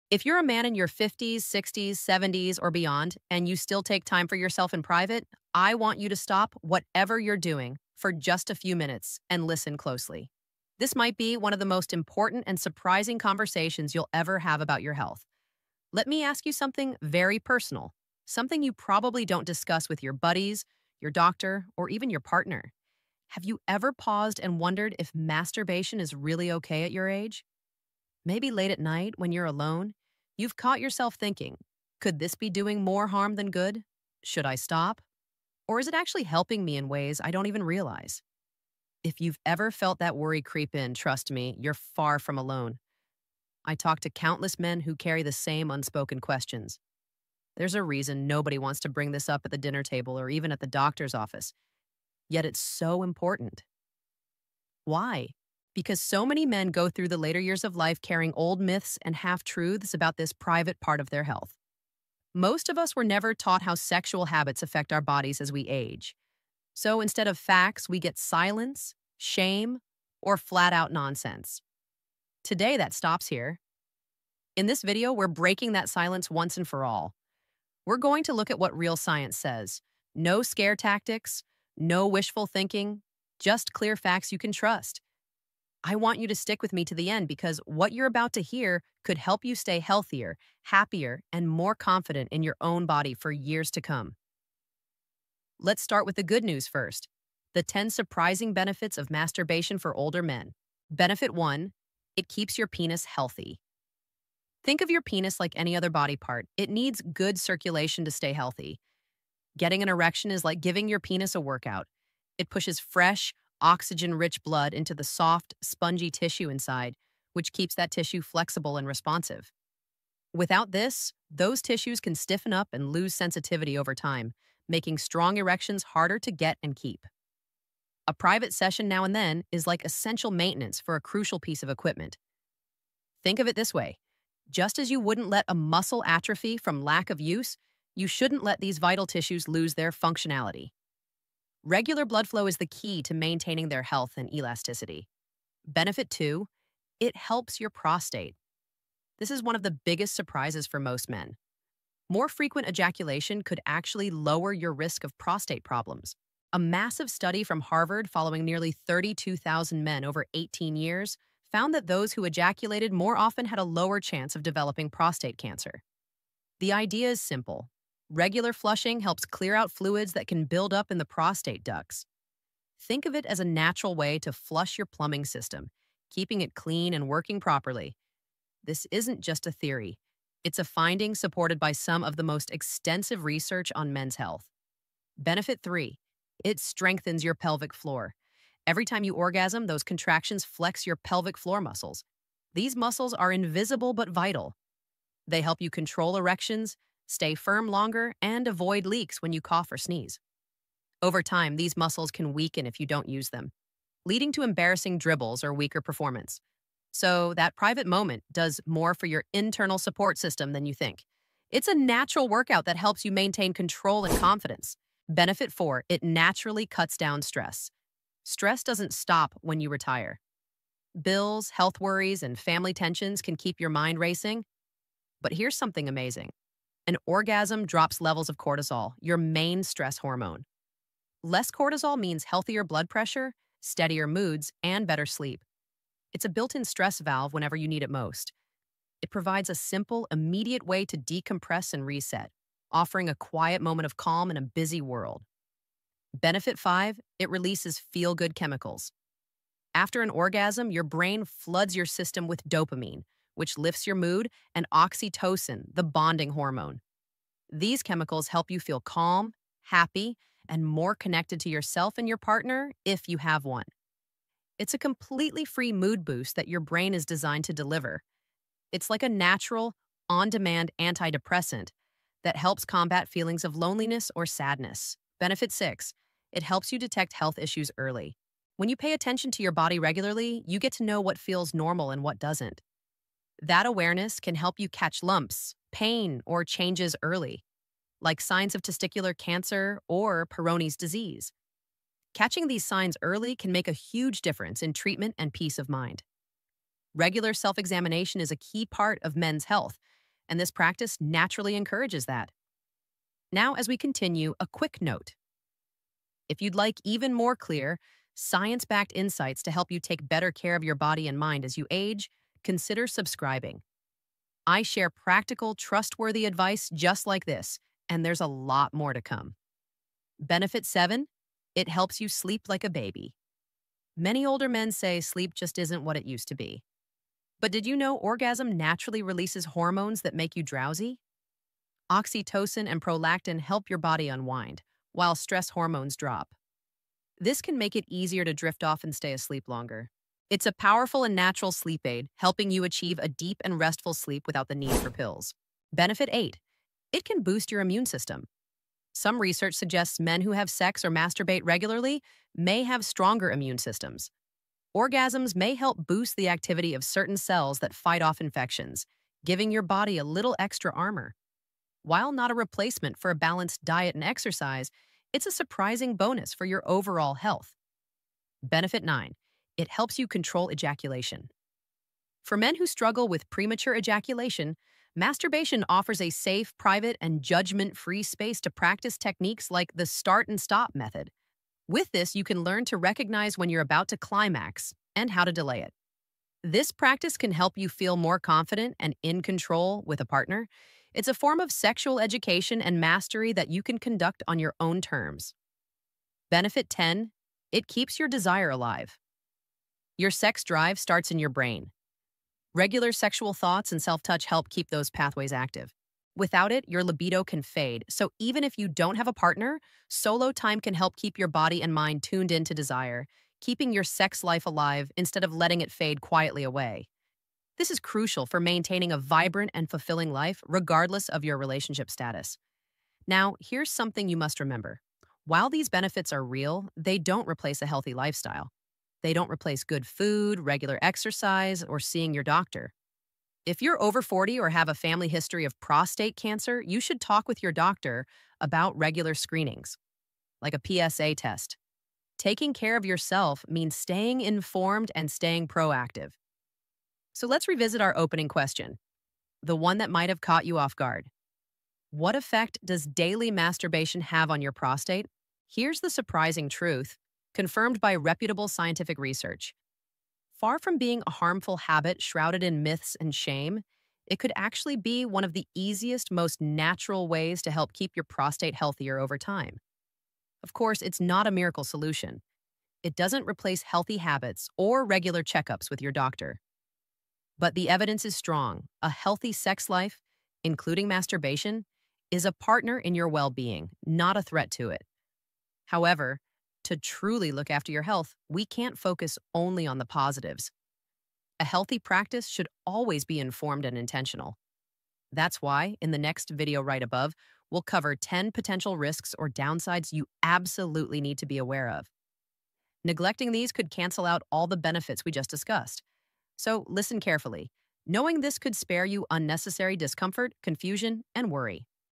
If You're Older And MASTURBATE, I Ask You To Watch This Video... | Urologist explains
This video reveals the 10 surprising benefits of masturbation for older men—including prostate health, stress relief, better sleep, and stronger erections—backed by real science and men’s health research.

If you’re a man in your 50s, 60s, 70s, or beyond, this step-by-step guide uncovers the 10 powerful health benefits of masturbation for older men. Far from being harmful, solo activity can actually support your prostate, improve erections, boost mood, and even strengthen your immune system. In this video, we’ll break down each benefit in clear, science-backed detail—helping you take charge of your health, confidence, and sexual wellness.

By the end of this video, you’ll understand:

Why masturbation keeps your penis healthy and improves blood circulation.

How regular ejaculation can lower the risk of prostate problems, including prostate cancer.

The surprising connection between orgasm, stress relief, and better sleep.

How pelvic floor strengthening boosts control and confidence.

The role of dopamine, oxytocin, and other feel-good chemicals in mood and sexual desire.

Why masturbation helps detect health issues early and supports men’s overall health.

How solo practice can improve control over ejaculation and keep libido strong.

Whether you’re concerned about prostate health, stress, sleep, or simply staying confident as you age, this video gives you the truth about masturbation for older men. No myths, no shame—just facts you can trust. Take care of your body, protect your sexual health, and discover how this natural practice can keep you healthier, happier, and more energized at any age.

It discusses human anatomy, men’s health, and sexual function from a medical and scientific perspective.
It is not intended to be sexually suggestive, explicit, or gratifying in any way.
The presenter shown is a digital avatar used to visually explain medical concepts.
No real people are depicted or involved in any sexual context.
All visuals are designed for health education and viewer understanding.
This content does not replace professional medical advice, diagnosis, or treatment.
Always consult a qualified healthcare provider regarding any medical or health concerns.